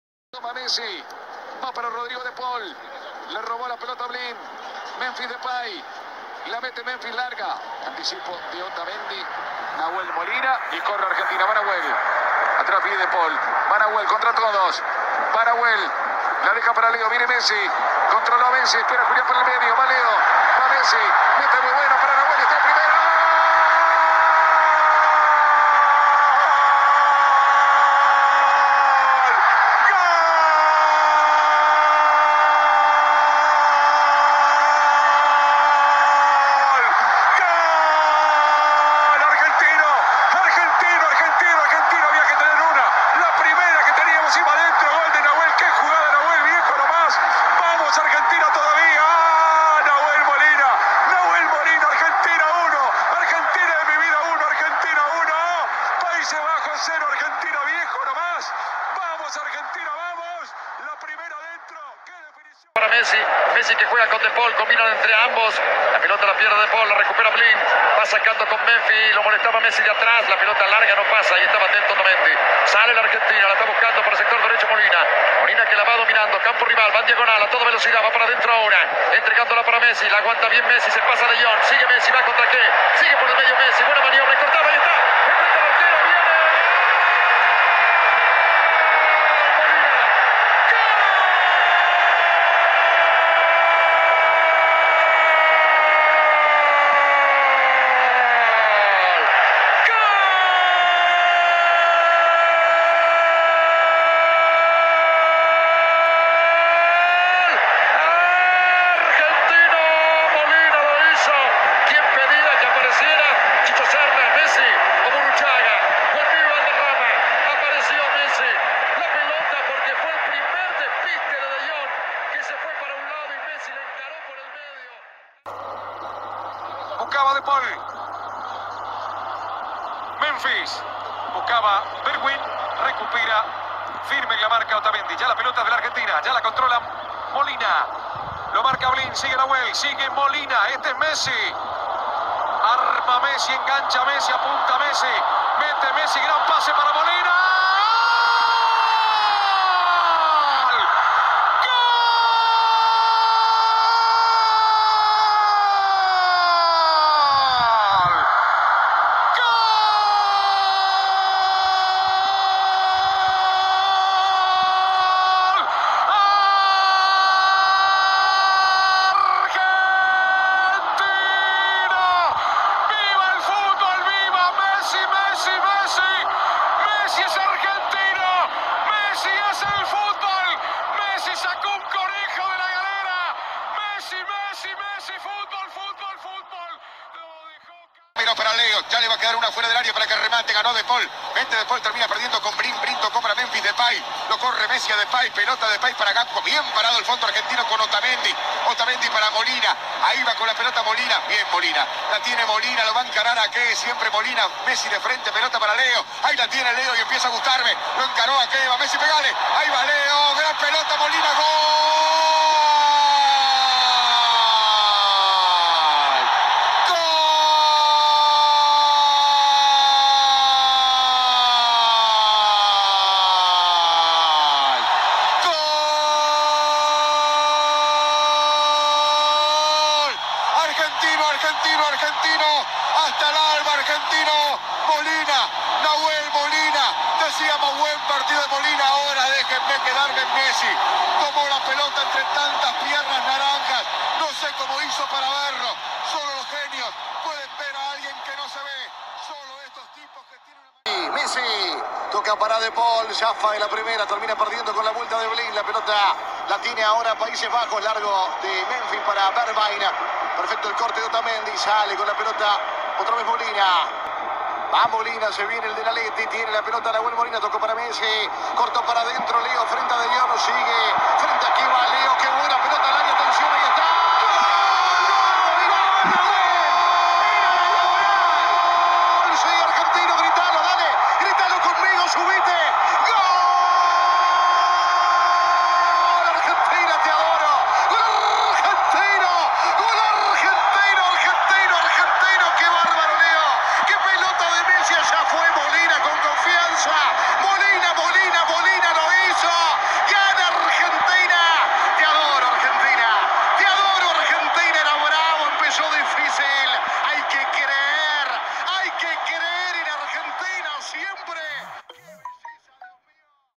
Gol de Nahuel MOLINA vs Paises Bajos | Relatos ARGENTINOS | Qatar 2022
🇦🇷 Argentina vs Paises Bajos 🇳🇱 [2-2]
4tos De FINAL | Mundial Qatar 2022
🎙Relatos Argentinos:
▪Pablo Giralt
▪Mariano Closs
▪Rodolfo de Paoli
▪Sebastian Vignolo
▪Gustavo Kuffner